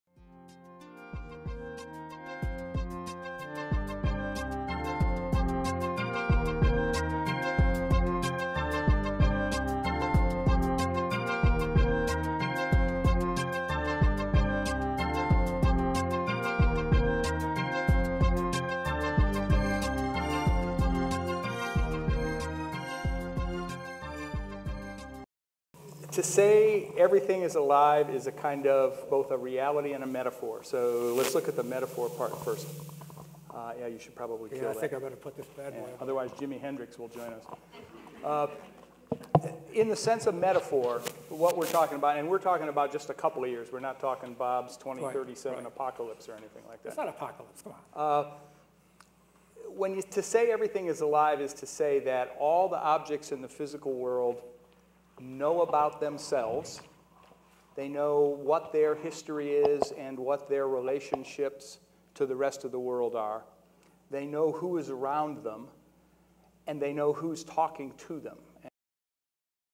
Everything's Alive/The Discussion Begins
A brief discussion as to why Everything's Alive is eloquently explained by Murry Christensen. There is no doubt things will be radically different from now on as we move further into the 21st Century.